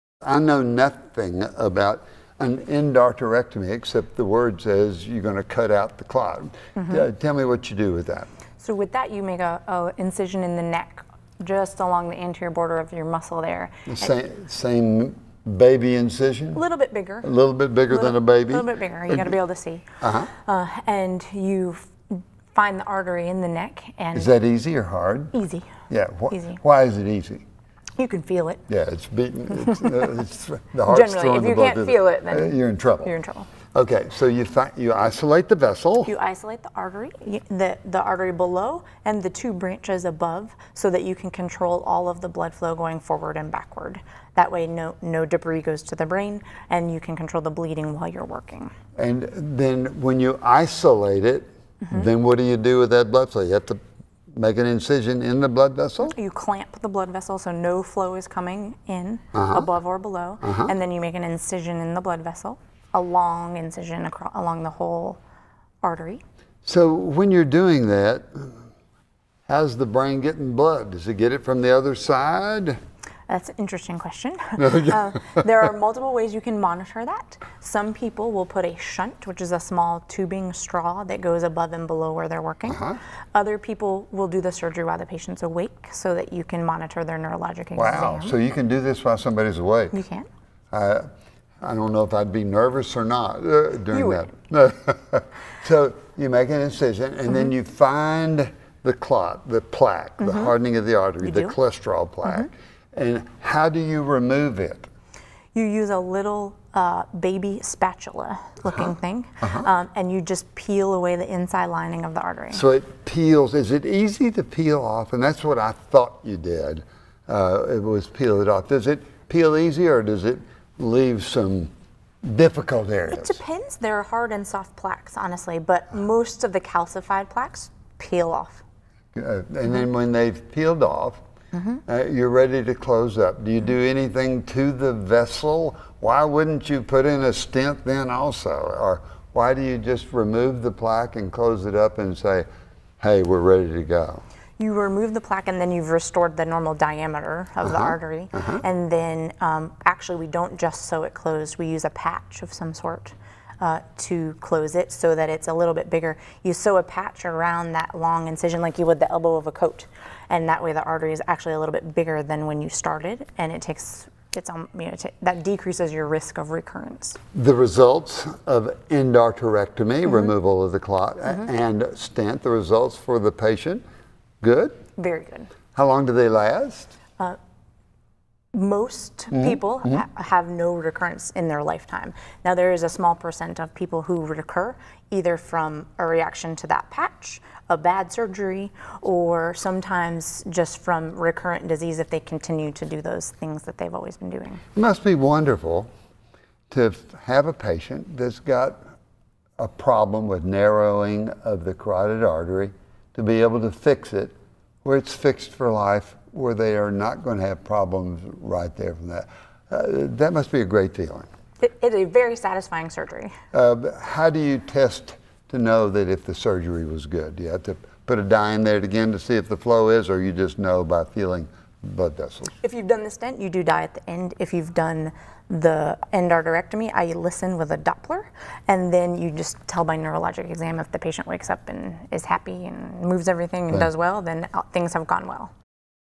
Endarterectomy Surgery for Carotid Artery Disease
Dr. Deanna Nelson, Board Certified Vascular Surgeon, explains another surgery option besides stents to help with carotid artery disease.